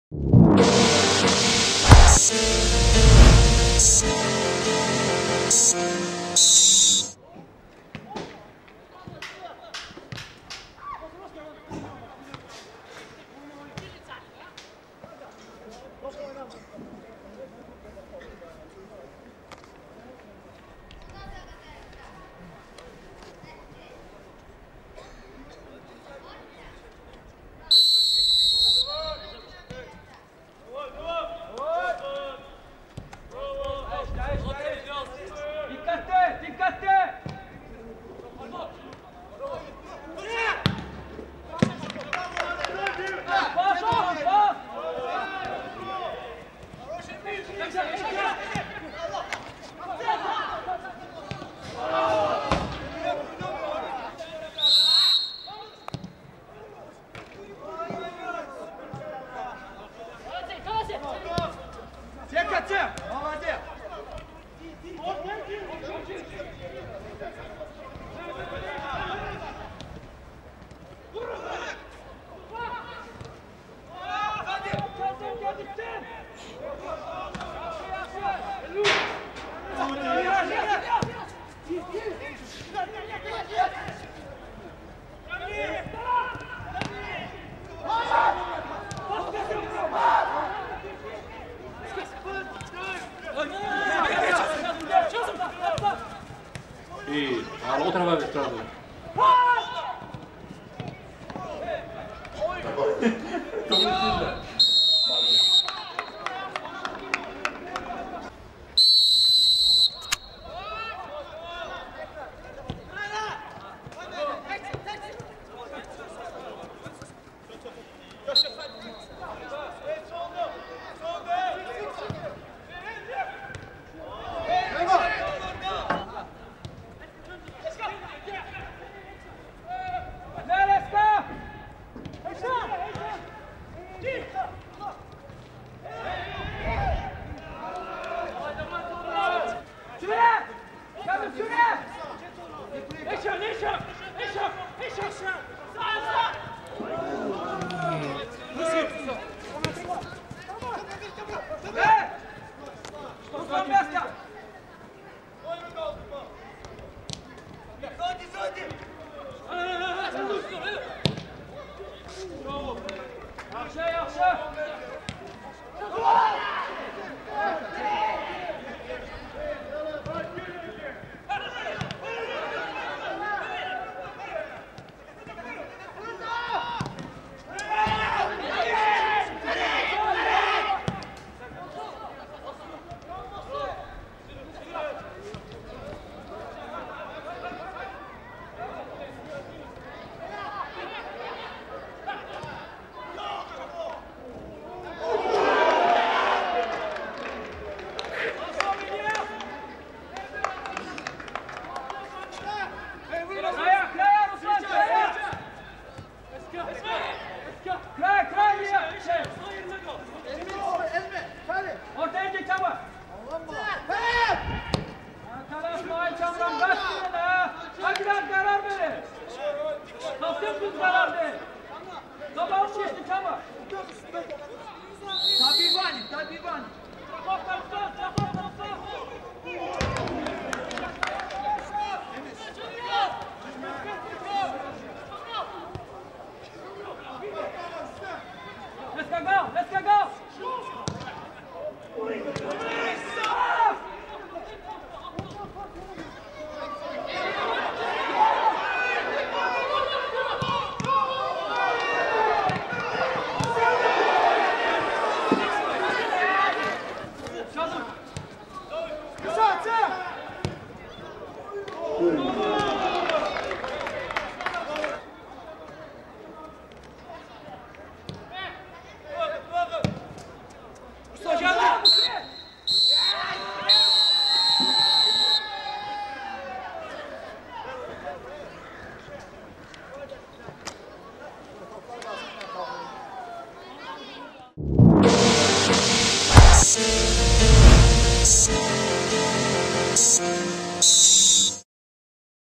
Əvəzedicilər: "Sumqayıt" - "Xəzər Lənkəran" 0:0 (İcmal)
Əvəzedici heyətlərin çempionatı, 2014/15 mövsümü, X tur.
Matçın icmalı.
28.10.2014